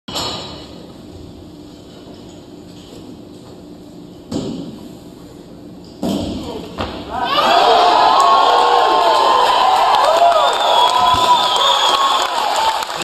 Casey Burgener Junior American Record 165kg Snatch
Casey snatching a Junior American Record 165kg in the 105kg class at the 2002 American Open in Savannah, Georgia.